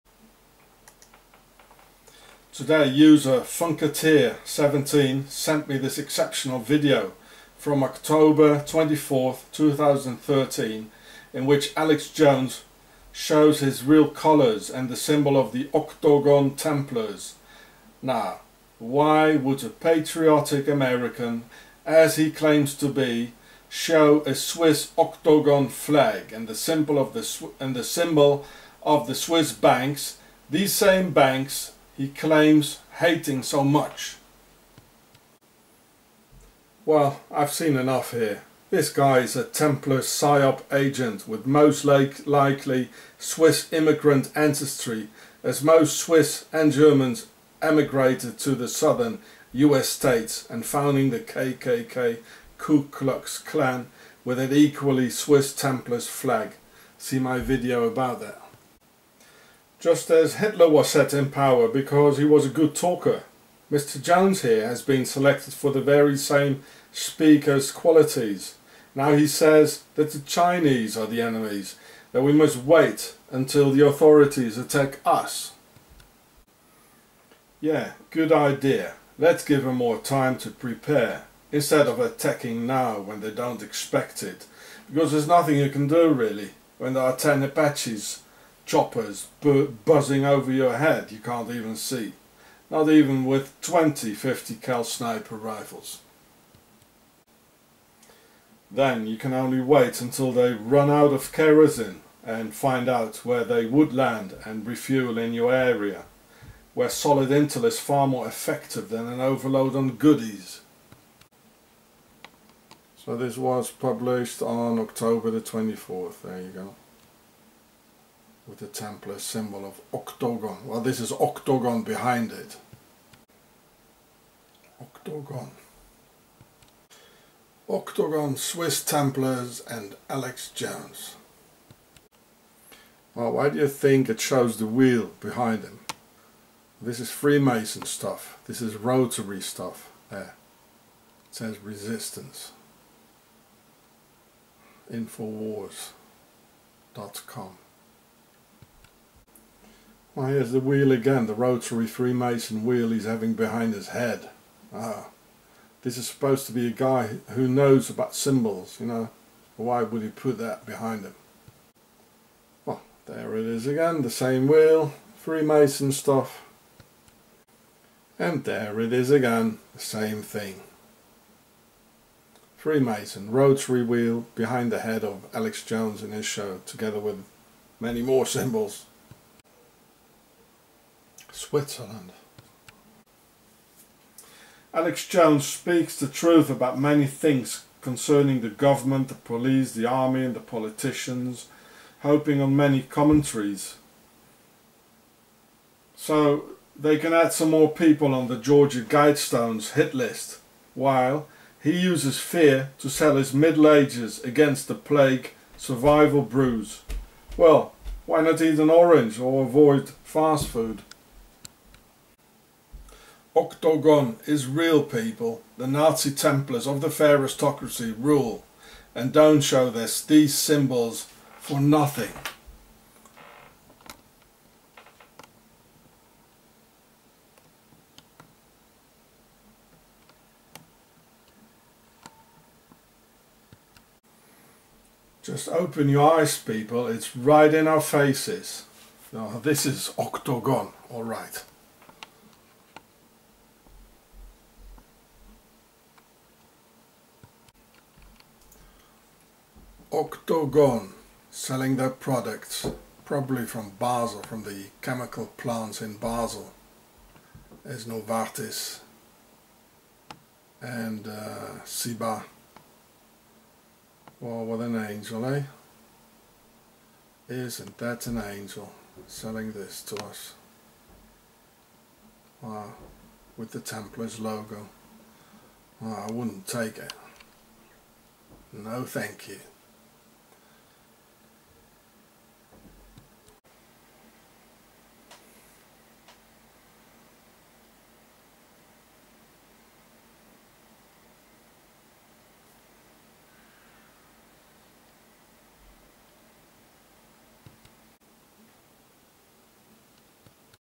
Who is Alex Jones really? Infowars their Swiss Bank Octogon Knights Templars Rotary Freemason Logo
Watch "Octogon The Empire of Darkness", "Pharistocracy" and "The Pharaoh Show".

OCTOGON AND THE SWISS NAZI TEMPLARS OF THE PHARISTOCRACY RULE THE WORLD, OWN THE US THROUGH THE BIS, AND THEY INFILTRATE EVERYTHING BEFORE TAKING OVER CONTROL - THIS THING GOES REAL DEEP.  Apparently AJ is Bill Hicks, and apparently before that he was Alexander Christian Lafayette and a member of the Pharistocracy rumours say; these Pharaohs always change their names just like that, like "french" president Nicholas Sarkozy officially changed his name into Nico Sarcosi, when he was living in Corsica and had two children there with a corsican woman.

The user whom I gave the credits to this film just sent me this, so I`ll do, what he asked me for:

" Wow. I'm getting a bit worried now, as I know what these "people" are capable of. Is there any chance you could delete my name from the details underneath the video? Also, are you able to delete any of the comments that mention me as well? "